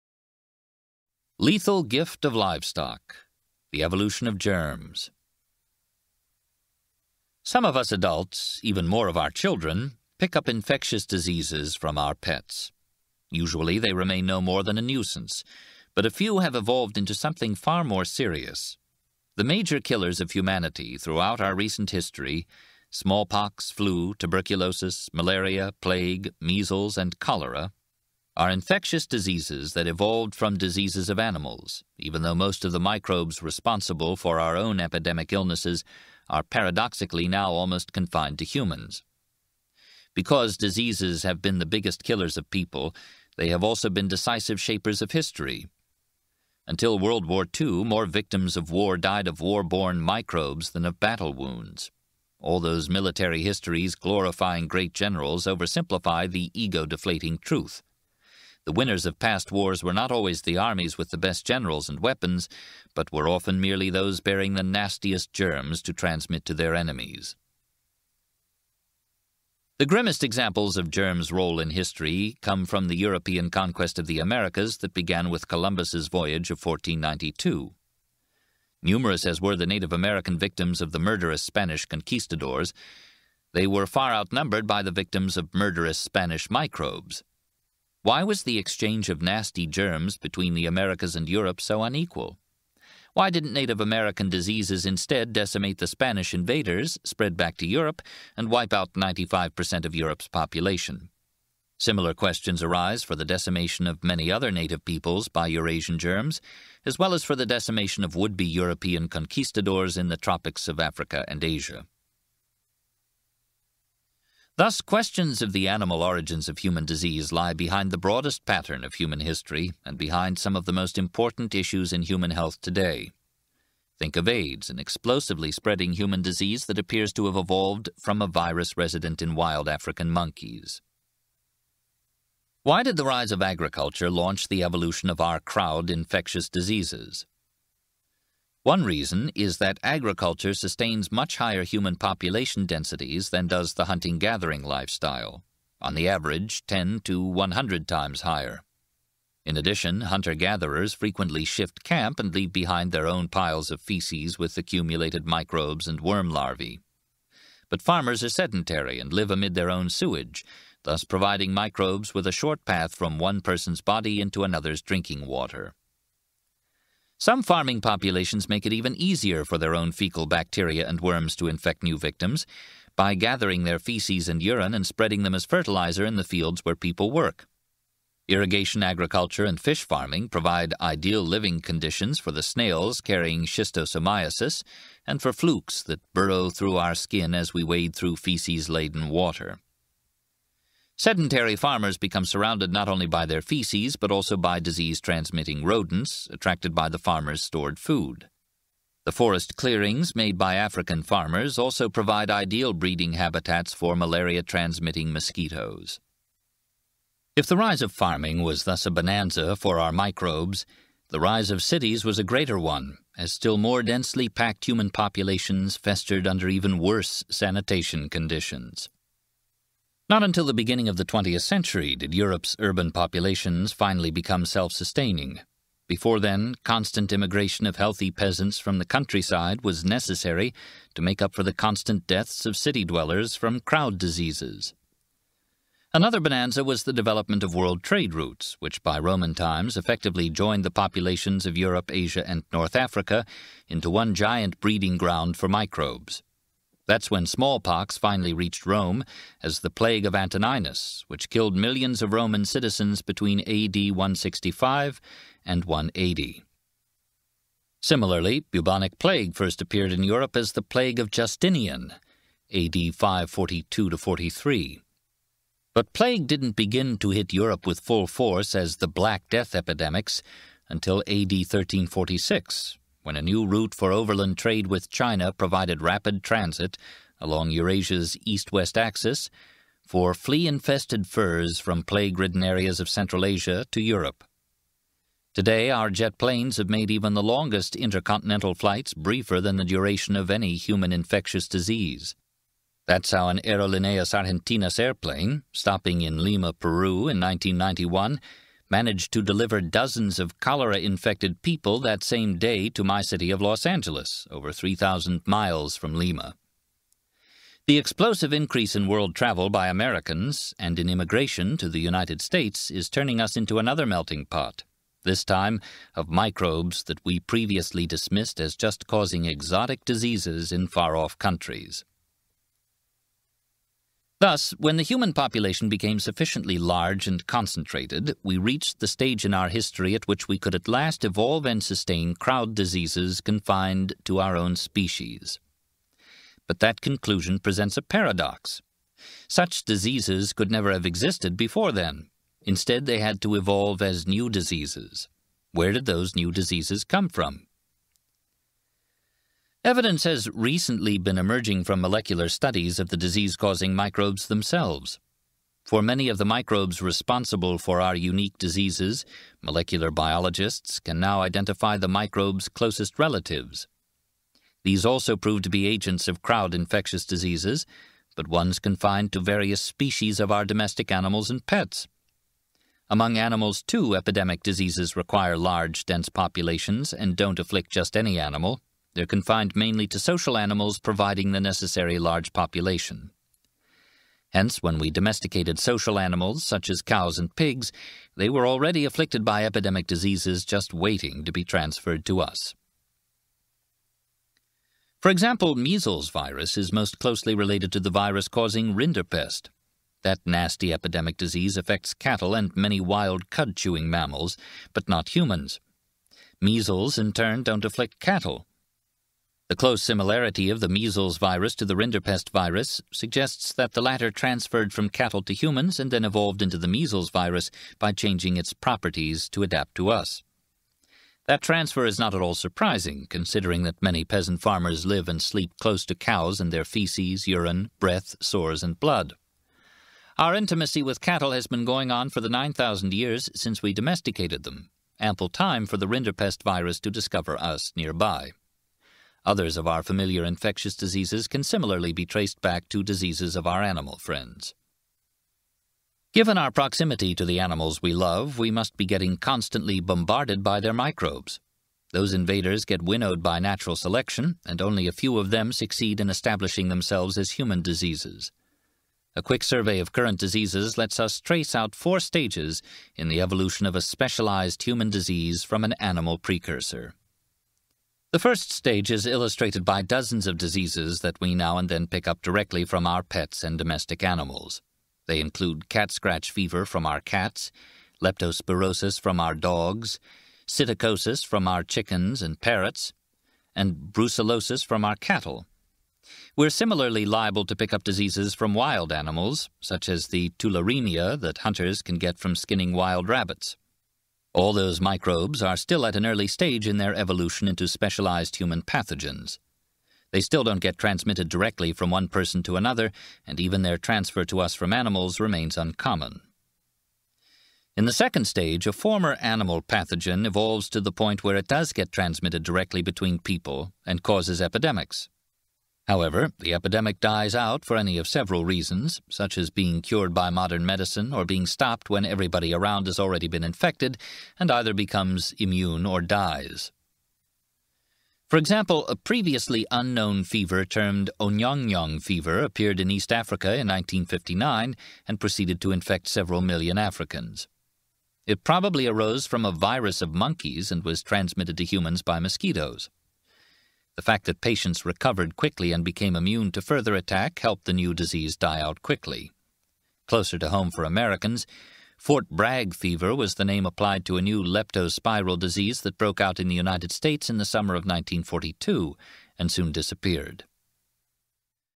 Guns Germs & Steel - Predicting The Global Pandemic [Jared Diamond]
Diamond talks about how Viruses Spread Historically And How Advances In Global Travel Could Have Devastating Effects For The Planet.